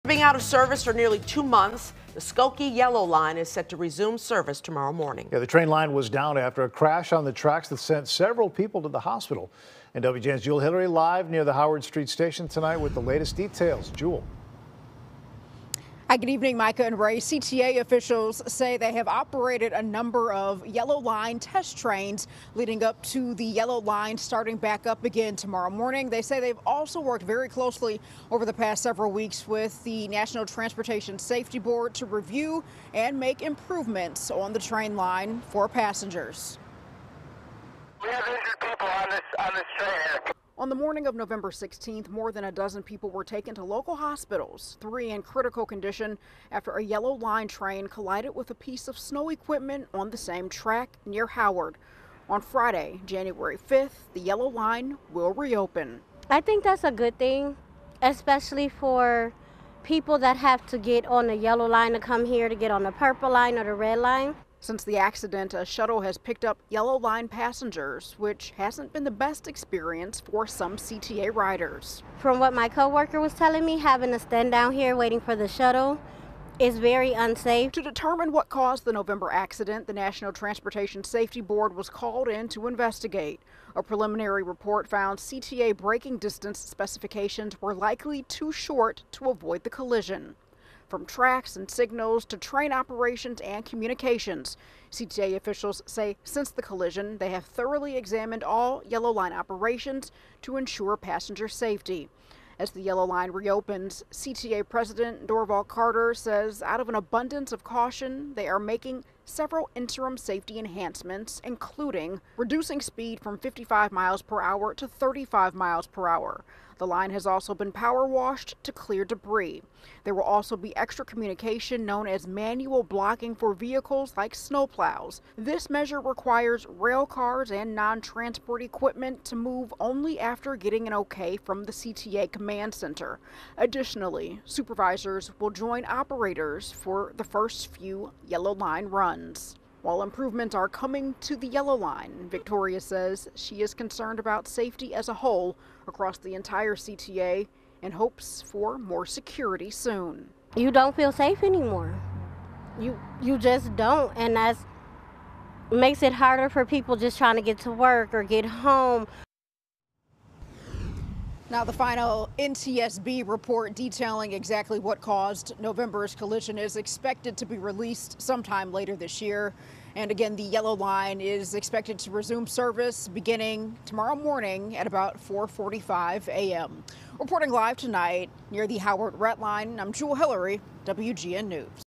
Nearly 2 months after train crash, CTA Yellow Line set to re-open
Nearly two months after a CTA Yellow Line train crashed into a piece of snow removal equipment, sending more than a dozen people to the hospital, train service is set to resume, the transit agency said. WGN's Jewell Hillery reports on the story.